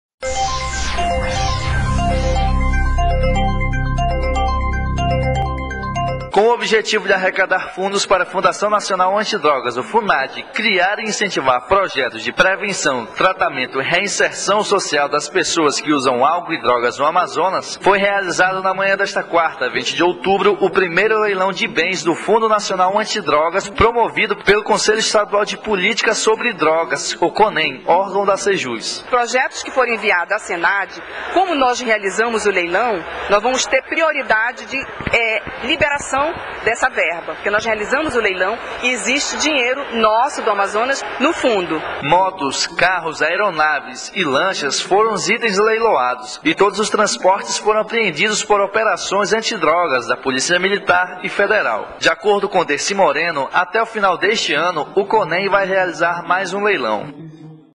CONEN realiza leilão com transportes apreendido pela polícia no combate ao narcotráfico
Foi realizado na manhã desta quarta, 20 de outubro o 1 leilão de bens do fundo nacional antidrogas.